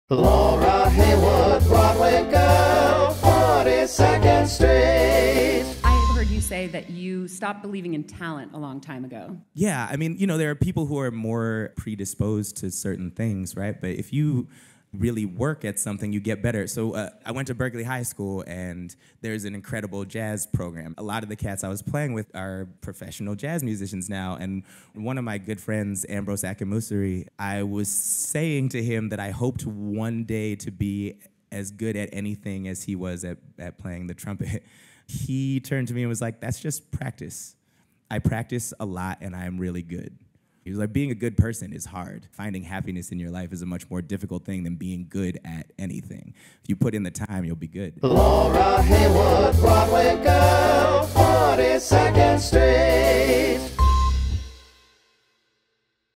40 Second St - DAVEED DIGGS on practice vs. talent
In just 40 Seconds, Daveed Diggs explains what his high-school friend Ambrose Akinmusire -- one of the world's greatest jazz trumpeters -- taught him about practice vs. talent.

For the full Daveed Diggs interview with Laura Heywood (@BroadwayGirlNYC) at AOL BUILD